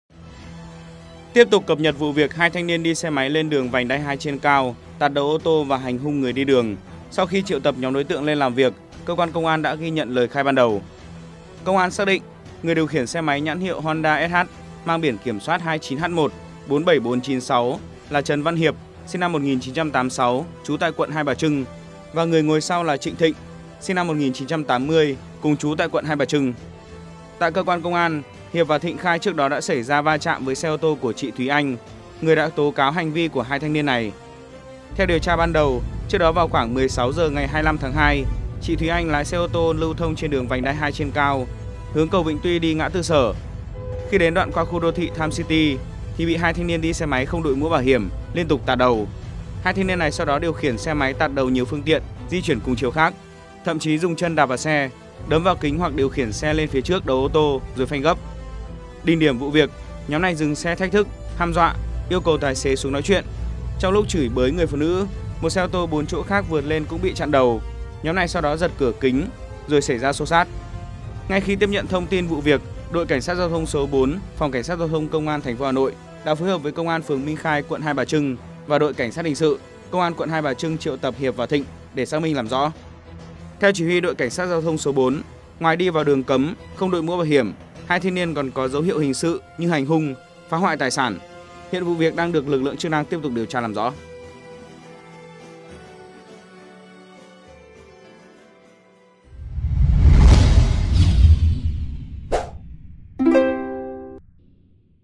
Hai thanh niên đi SH c.à k.h.ị.a, t.ấ.n c.ô.n.g người trên đường Vành đai 2 khai gì? |VTC14 GREEN
VTC14 GREEN| Hai thanh niên đi SH cà khịa, tấn công người trên đường Vành đai 2 khai gì?